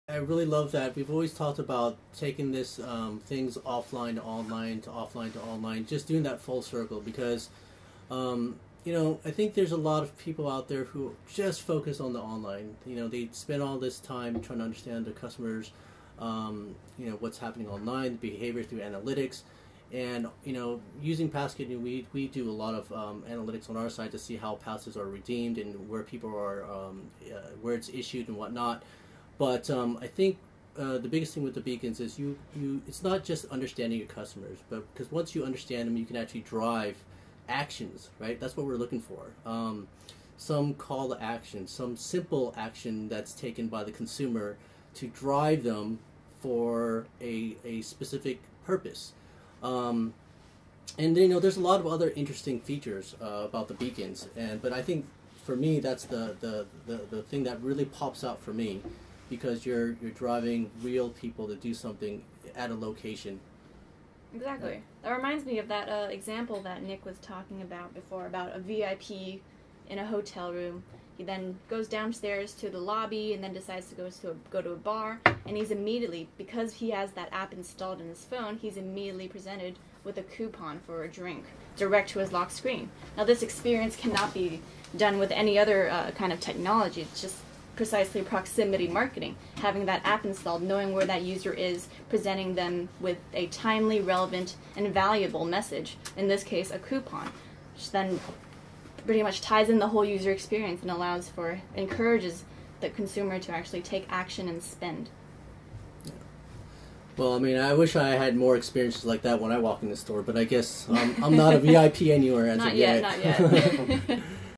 Creating Value Through Contextual Marketing with iBeacon
Leverage beacon technology to deliver contextually relevant content to your customers. iBeacon allows you to engage with your customers and provide something of value to them at the right place and time.